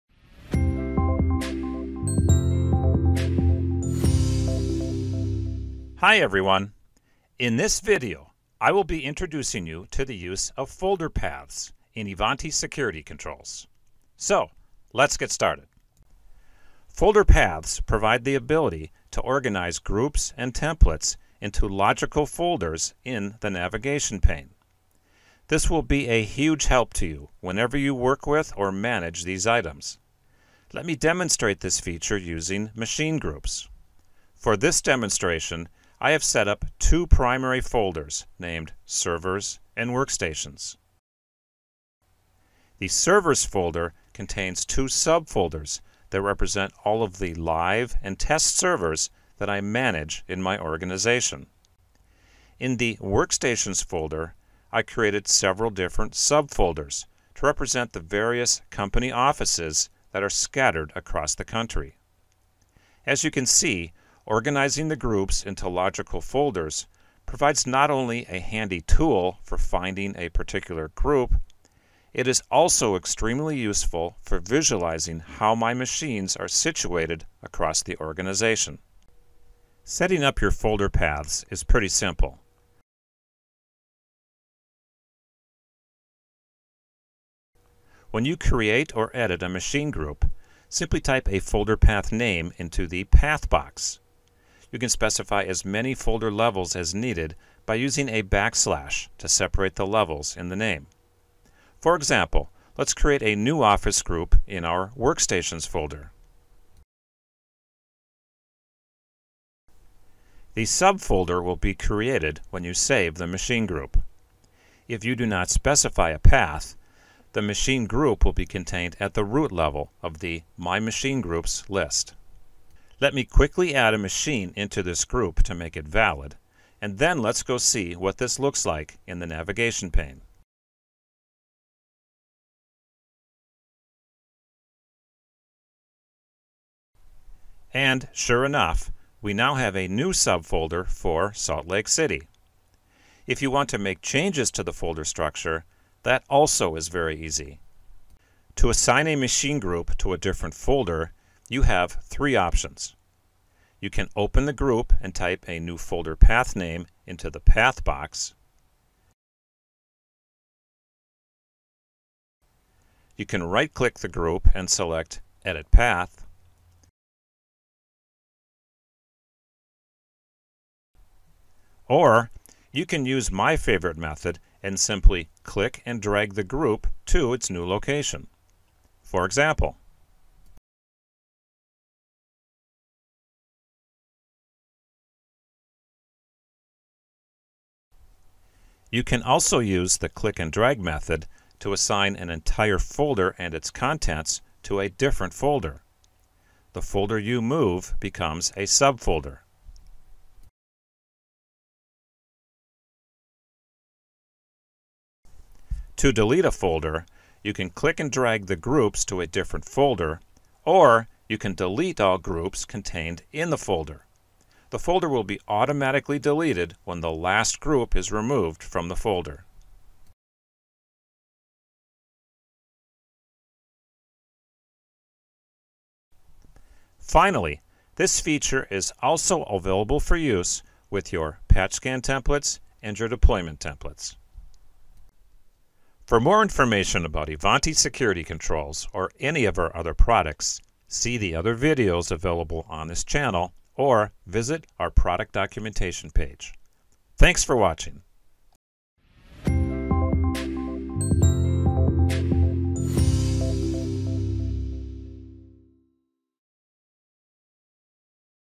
Using Folder Paths in the Navigation Pane
Folder paths provide the ability to organize groups and templates into logical folders in the navigation pane. This video demonstrates how to use this feature in Ivanti Security Controls.